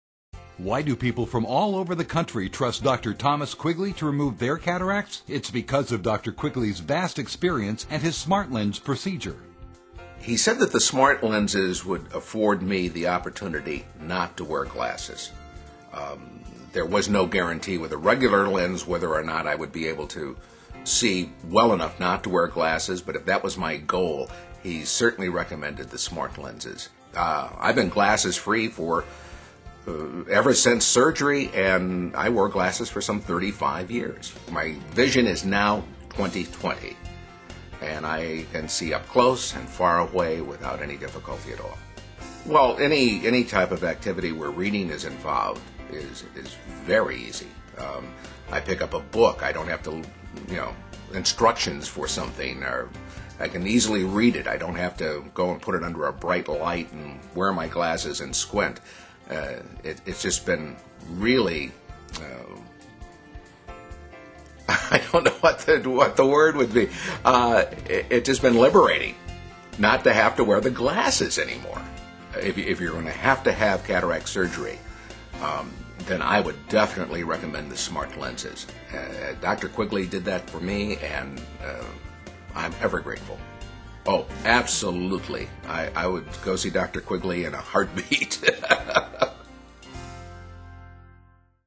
Eye Care Stories - Paul Williamson
After wearing glasses for more than 35 years, Paul's vision was worsening by a developing cataract. After an exam with Dr. Quigley that resulted in a SmartLens® procedure, Paul is now "liberated" from glasses and is enjoying 20-20 vision!  to learn more about how we can help you.